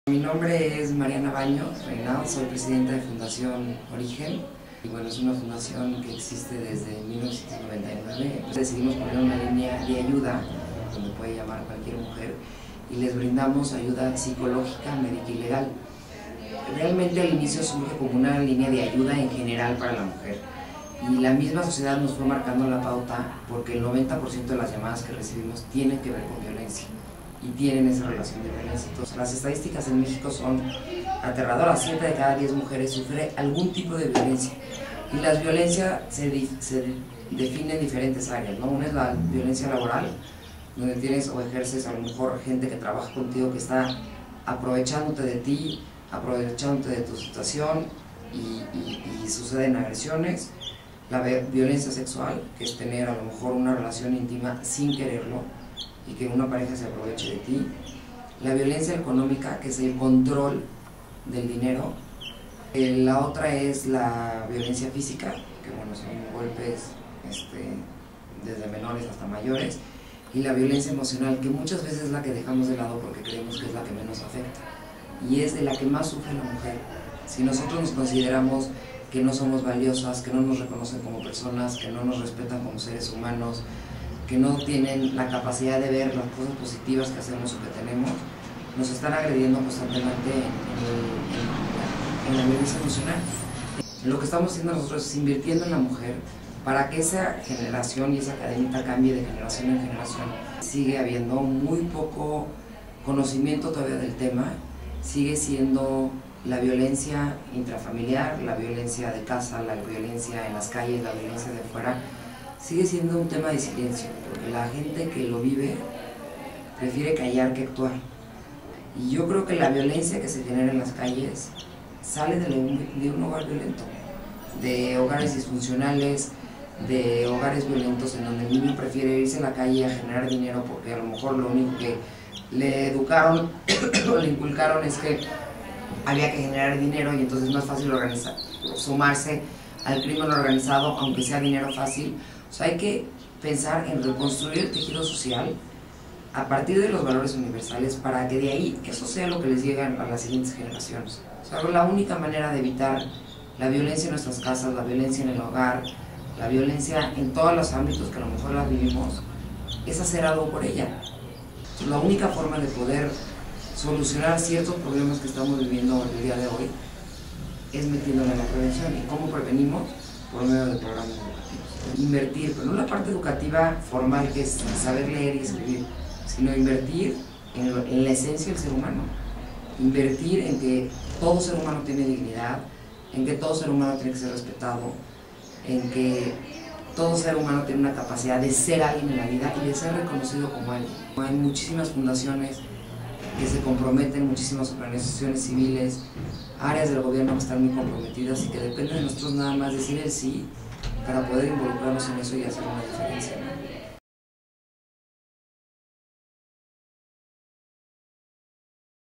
Entrevista Fundación Origen - Violencia contra la mujer
Entrevista con Mariana Baños, Presidenta de Fundación Origen en México para la ayuda de mujeres víctimas de la violencia.